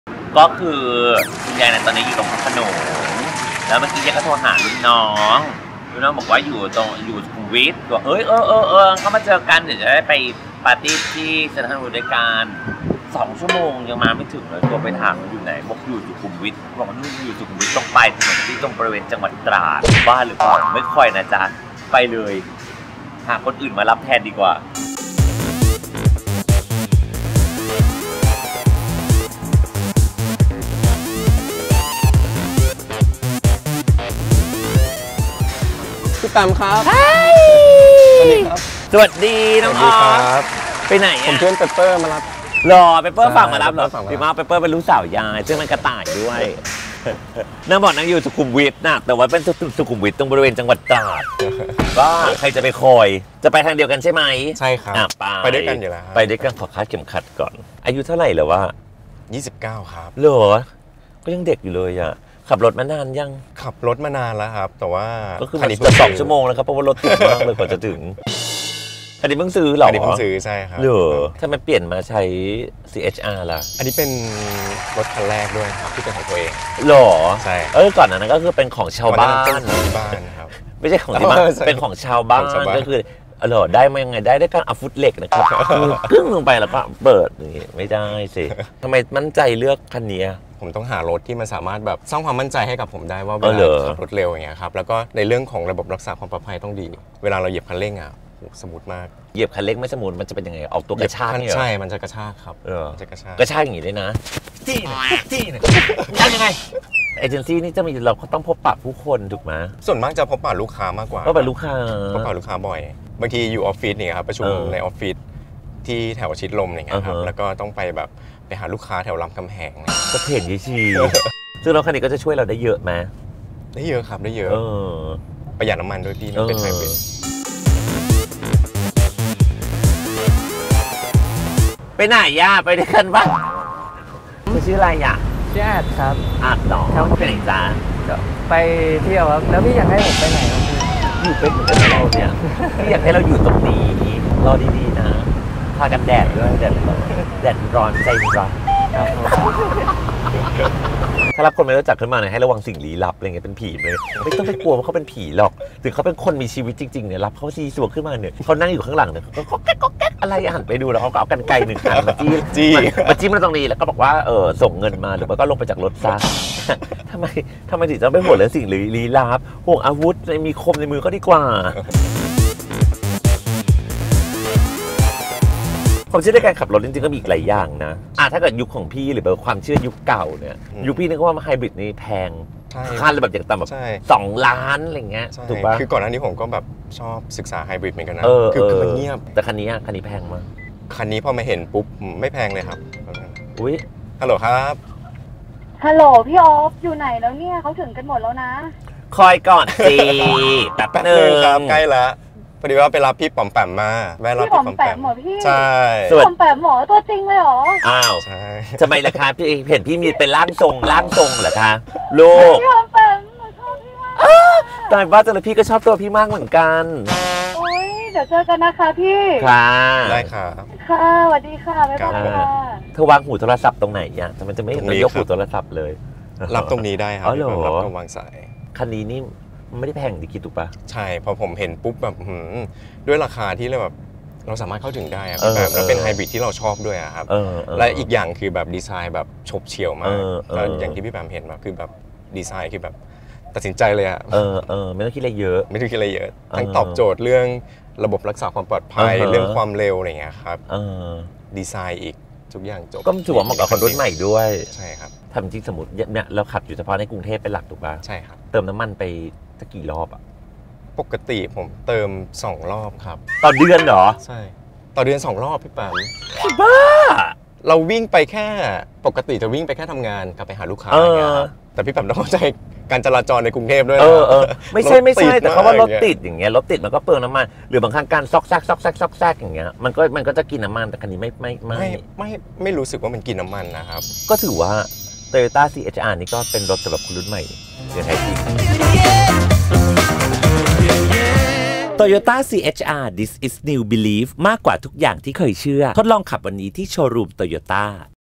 Real User x Toyota C-HR ตอนที่ 4 คุณอ๊อฟ มิเดียแพลนเนอร์
ชวนคุยกับคุณอ๊อฟ ผู้ใช้จริง C-HR HYBRID หนุ่มมิเดียแพลนเนอร์ ที่ต้องไปหาลูกค้าทั่วกรุงเทพ
ขับทุกวันในย่านรถติด ก็เติมน้ำมันแค่ เดือนละ 2 ครั้ง
ยิ่งได้ใช้ยิ่งชอบ HYBRID ดูแลง่าย ไม่เหมือนที่ใครๆ กังวลกัน แถมมีรับประกันให้ยาวๆ

นี่คือความเชื่อใหม่ ที่ผู้ใช้จริง C-HR ได้สัมผัสแล้ว
สัมผัสด้วยคุณเอง
ทดลองขับวันนี้ ที่โชว์รูมโตโยต้าทั่วประเทศ

TOYOTA C-HR มากกว่าทุกอย่างที่เคยเชื่อ